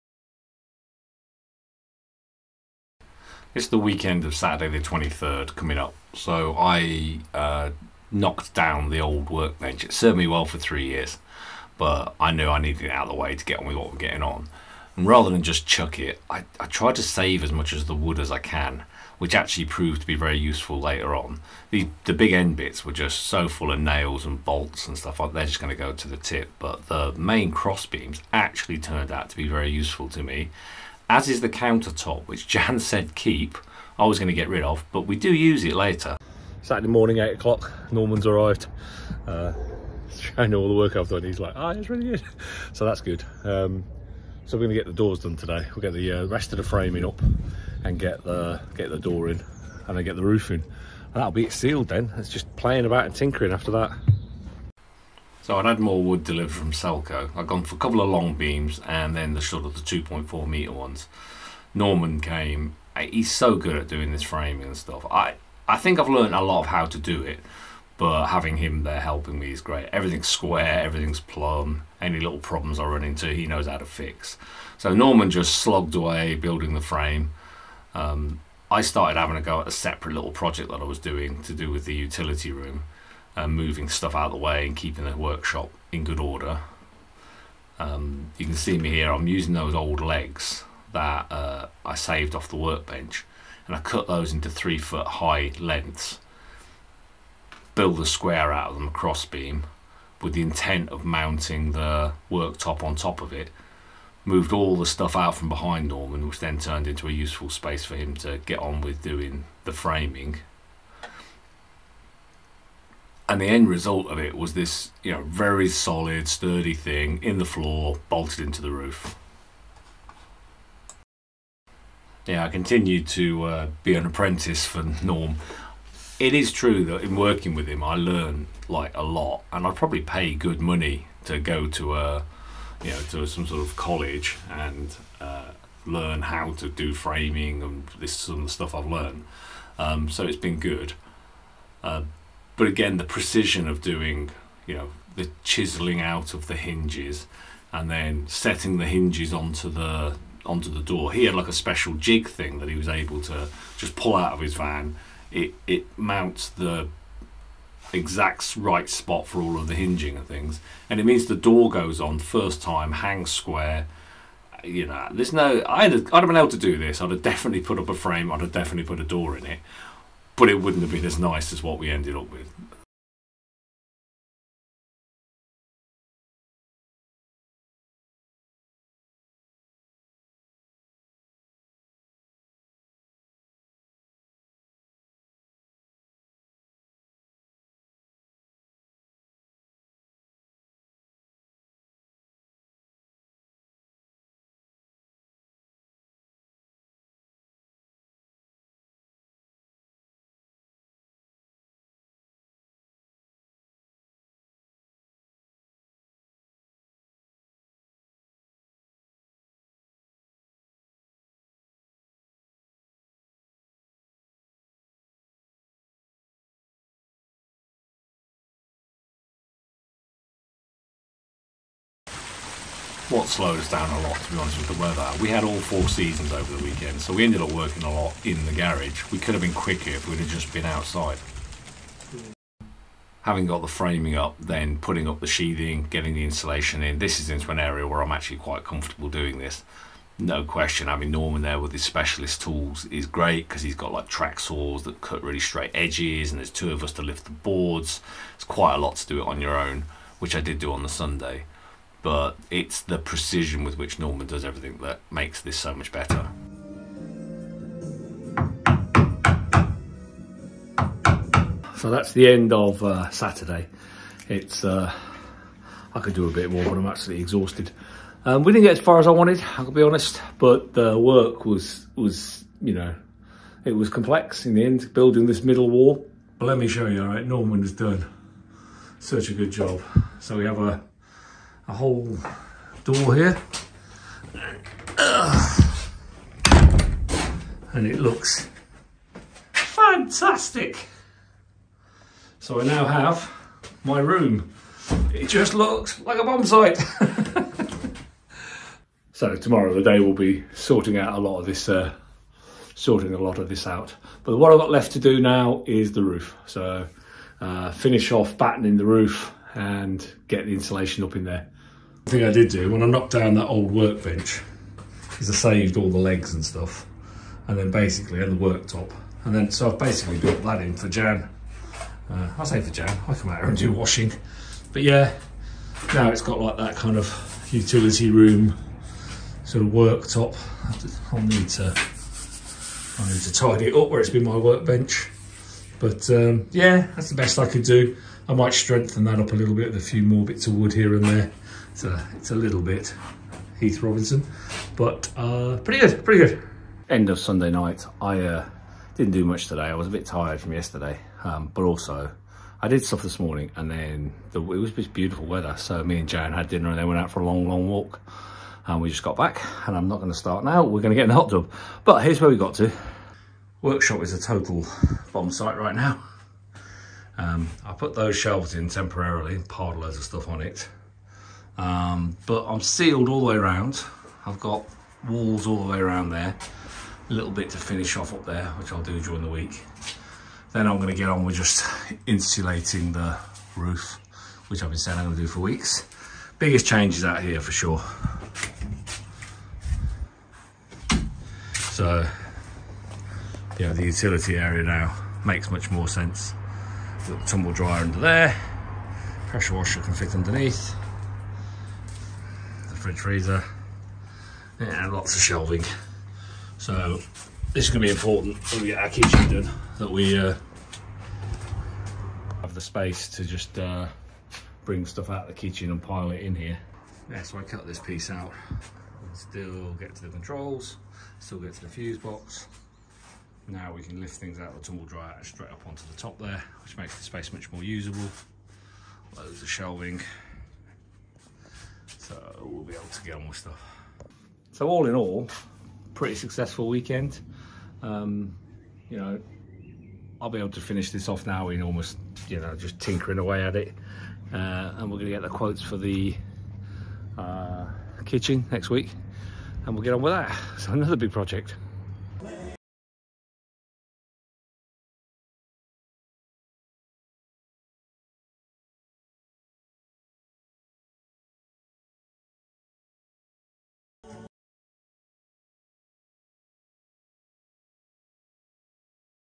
Workshop build part 4 - renovating the garage into a workshop
Broke the back of the work this weekend and we have two distinct rooms now.  The utility area is pretty much finished and that means we can store things there and they won't get covered in dust and paint.  Jan won't be tripping over stuff to get to the tumble dryer!

The workshop itself is a complete mess with tools and sawdust everywhere but I will chip away at that during the week.